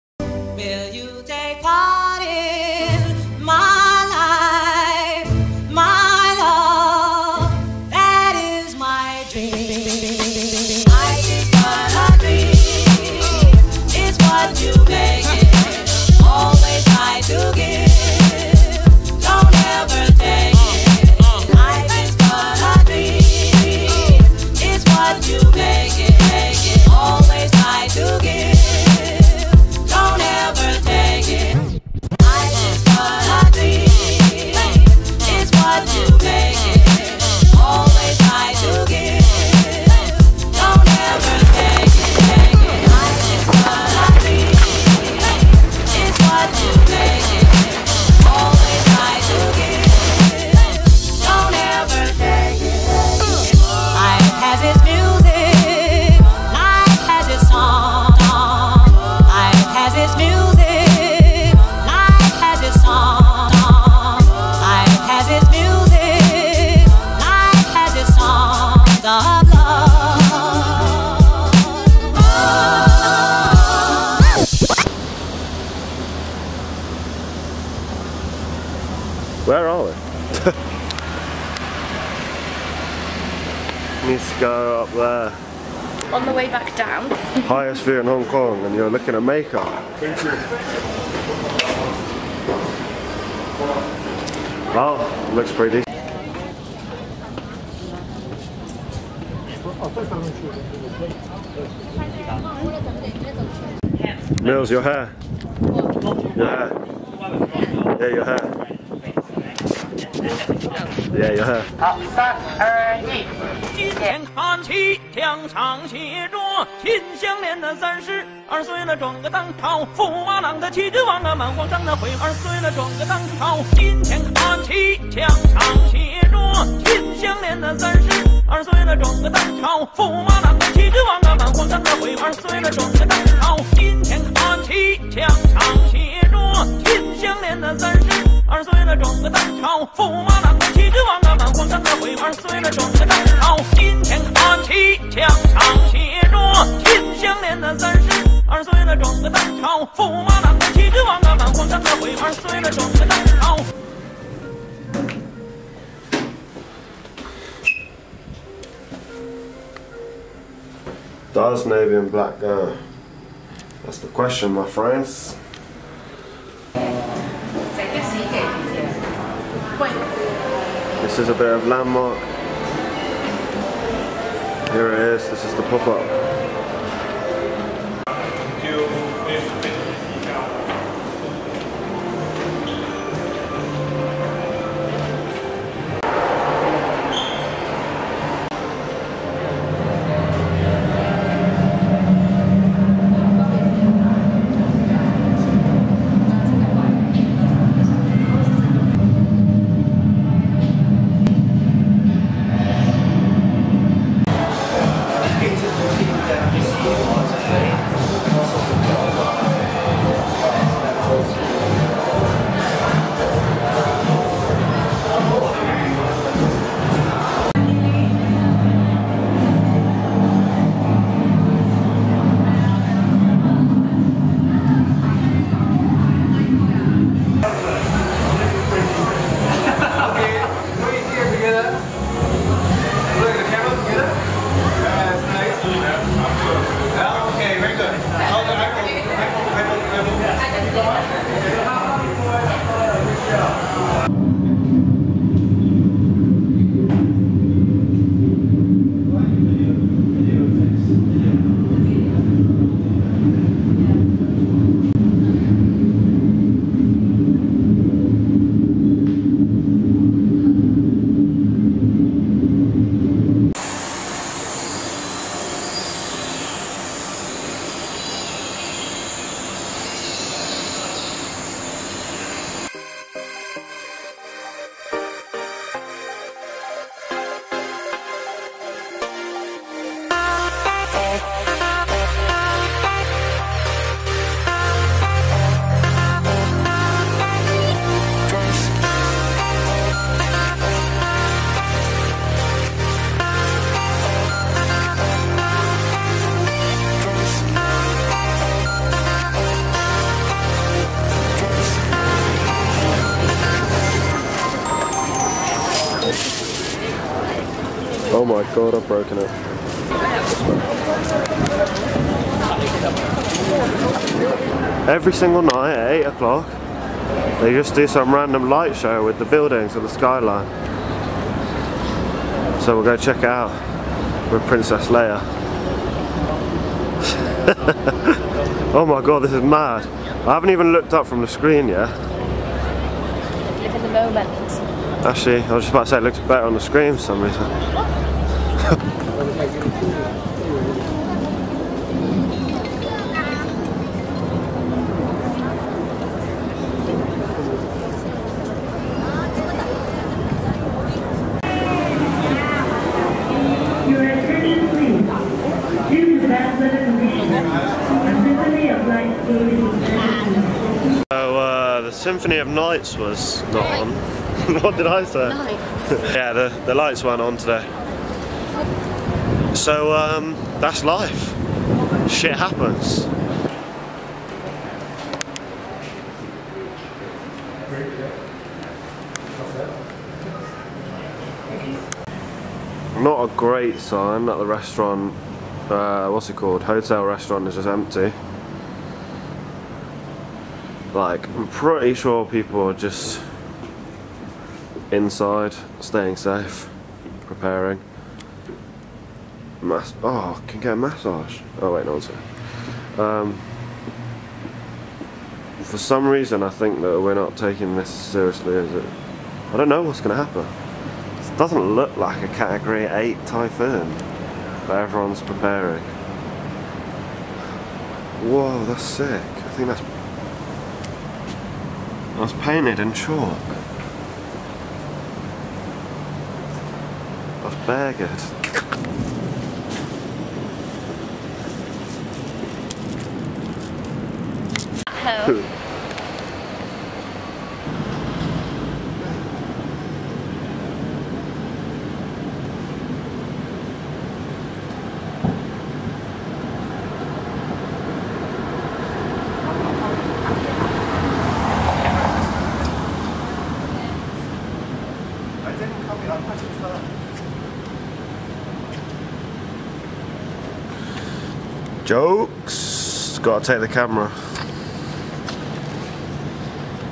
My 1st Solo Art Exhibition in Hong Kong
From London to Hong Kong to open and do some live painting in collaboration with Landmark HK.